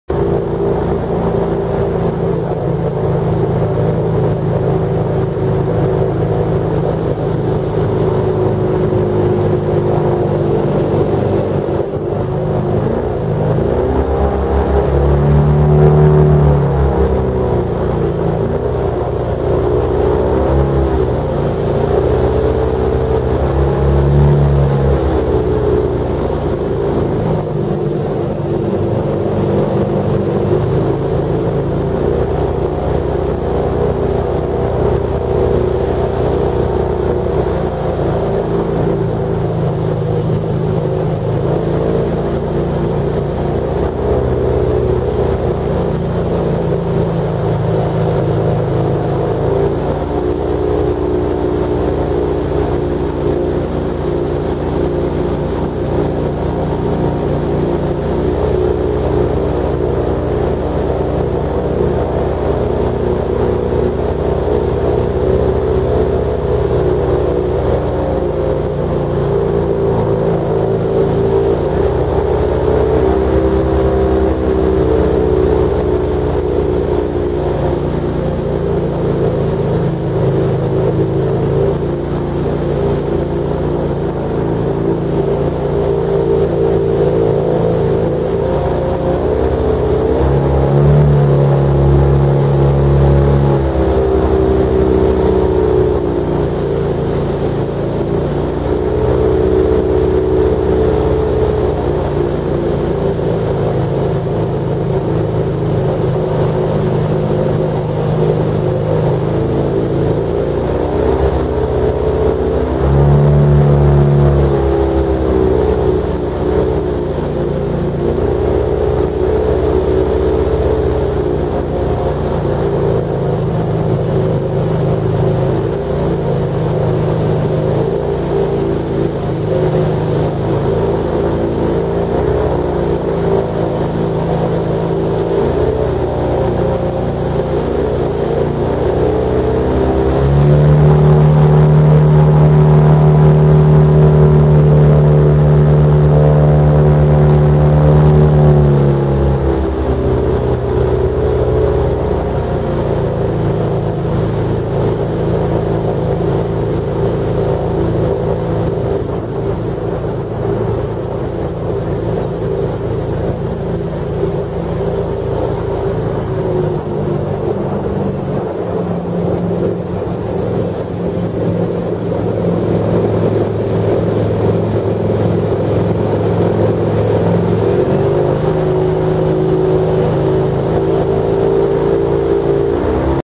Airboat in the Everglades (1 of 3)
Riding on an airboat in the Everglades of Florida.  January 2017.  Airboat was with Speedy's Airboats.